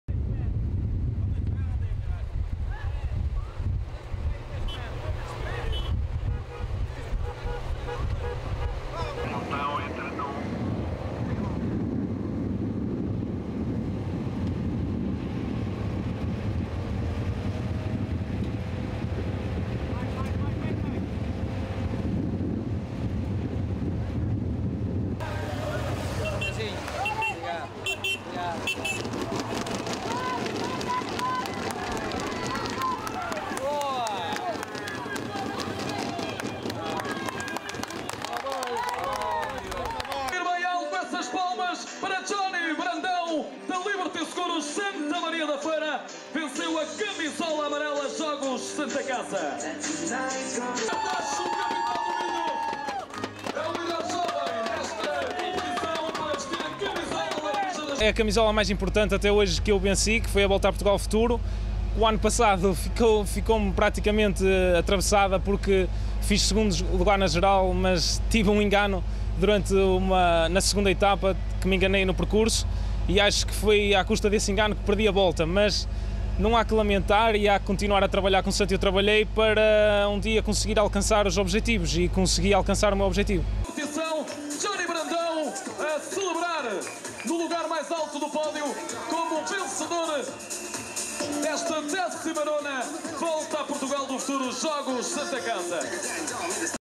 Do primeiro ao último dia Brandão sempre de Amarelo
Joni Brandão de 21 anos venceu este domingo a 19ª Volta a Portugal do Futuro Jogos Santa Casa. Depois de cinco dias de prova e cumpridas outras tantas etapas, o corredor e a equipa Liberty Seguro/Sta. Maria da Feira fizeram a festa em Alenquer. O triunfo de Brandão e do conjunto comandado por Manuel Correia começou a desenhar-se em Águeda, no final da primeira etapa quando a equipa alcançou as três primeiras posições e Joni Brandão a camisola amarela.